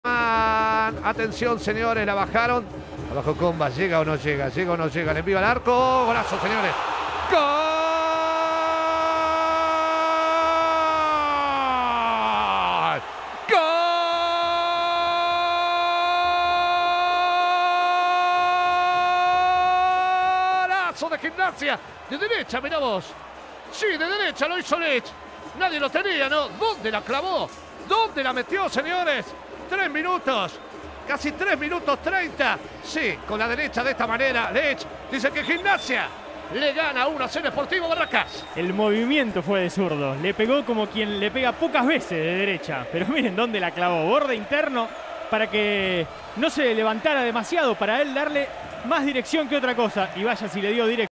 GOLAZO de Lucas Licht - Gimnasia LP 1-0 Sportivo Barracas - Copa Argentina 2020 32avos de final
GOLAZO de Lucas Licht para abrir el marcador para Gimnasia LP ante Sportivo Barracas, por los 32avos de final de la Copa Argentina 2020.